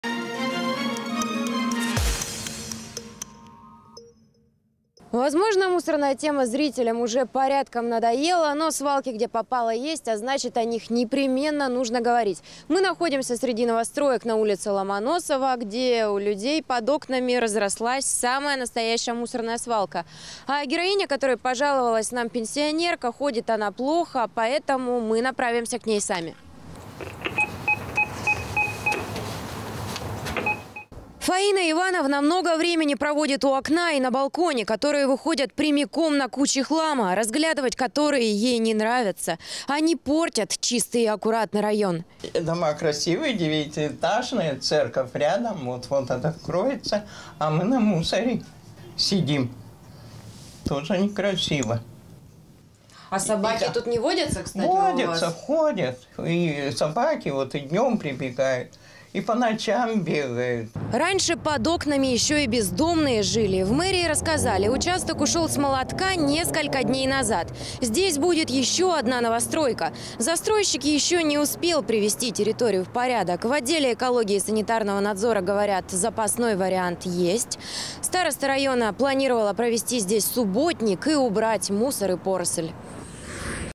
Кто уберёт мусорную поляну возле нового храма в Абакане
Здесь разрослась самая настоящая свалка.